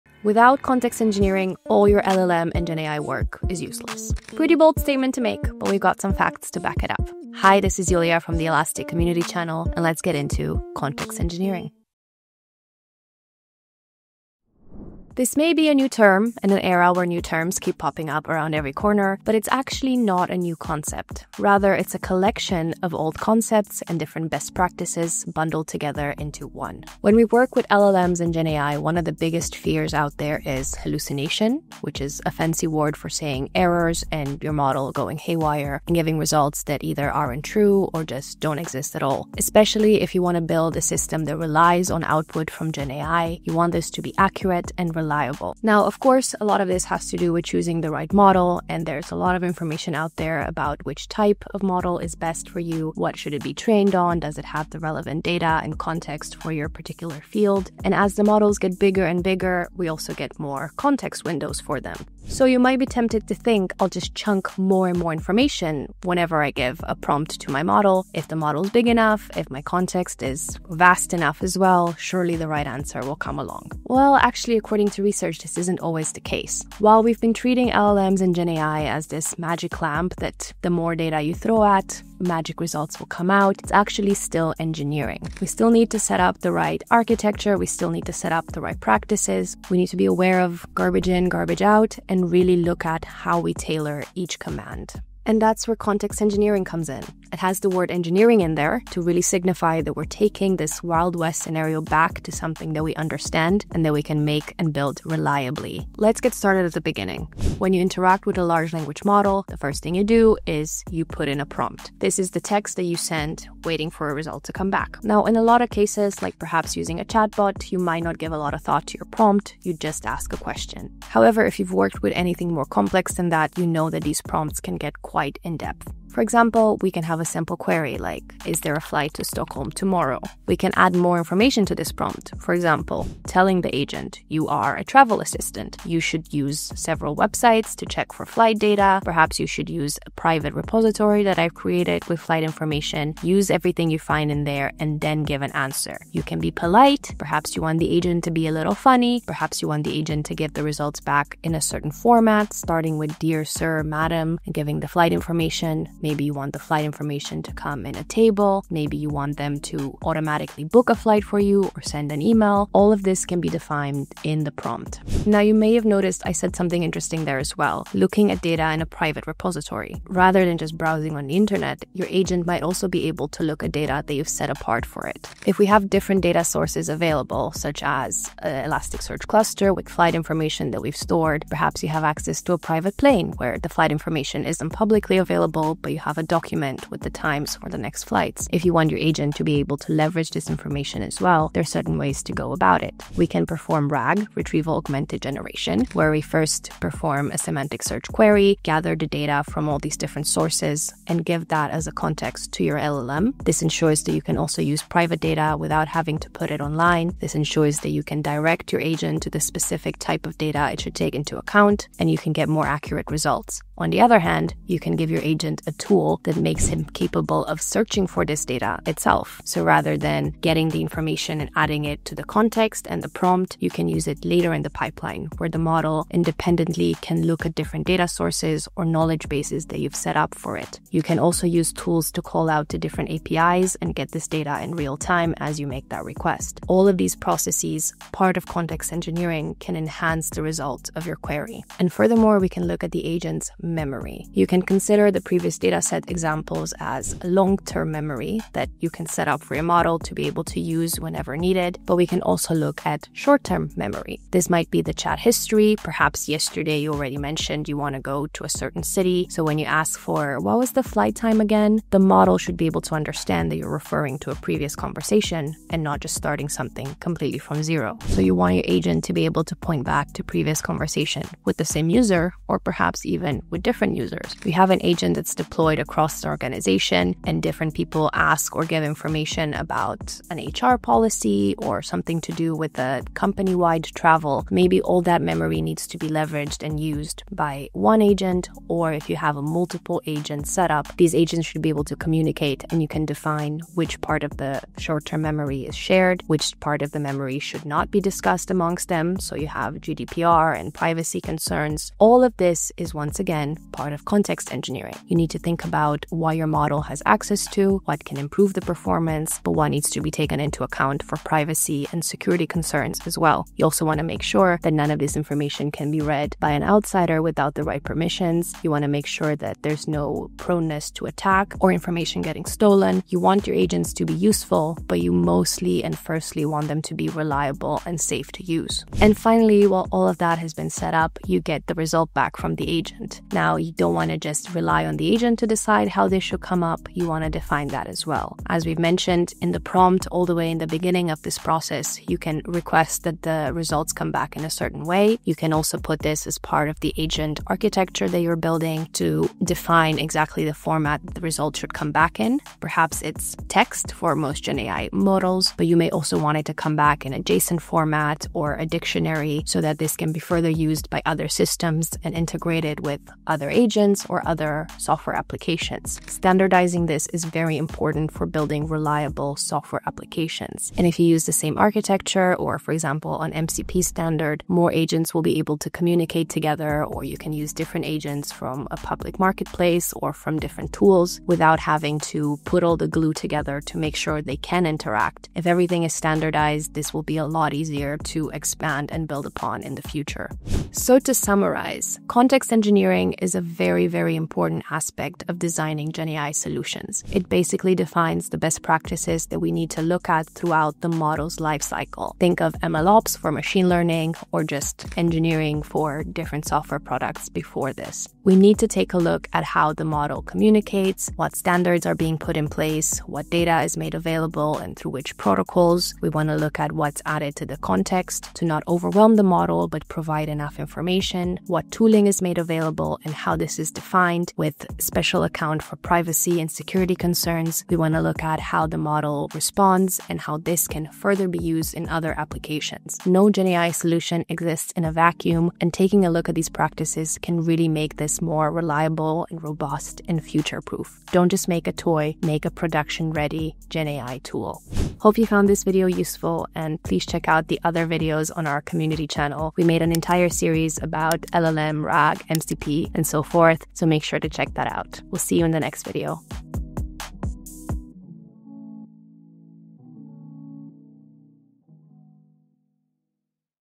Without context engineering, GenAI and LLMs are useless
In this video, we'll go over context engineering, the collection of practices used to define LLM and agent workflows. We’ll take a look at many topics, from prompt engineering, to data access (as well as privacy and security), RAG and retrieval strategies, context windows, and more. These practices will help you customize an agent for your task, increase performance and accuracy for your models, help you avoid hallucinations, and make your tool more scalable.

0:00  Why Context Engineering Matters
0:32  What Is Context Engineering?
1:10   Hallucinations & LLM Reliability
1:42   Why Bigger Context Isn’t Always Better
2:07   Importance of Prompt Design
2:33   Flight Bot Prompt Example
3:17   Using Private Data with GenAI
3:45   What Is RAG (Retrieval-Augmented Generation)?
4:20   Agents Using Tools & APIs
4:48   Memory in Agents: Short-Term vs Long-Term
5:25   Sharing Agent Memory + Privacy Concerns
6:05   Securing AI Systems from Attack
6:35    Defining Output Format (Text, JSON, etc.)
7:03    Why Standardization (MCP) Matters

Video created by: Iulia Feroli, Senior Developer Advocate @ Elastic

This video walkthrough is based on this Elasticsearch Labs article, which includes all necessary code snippets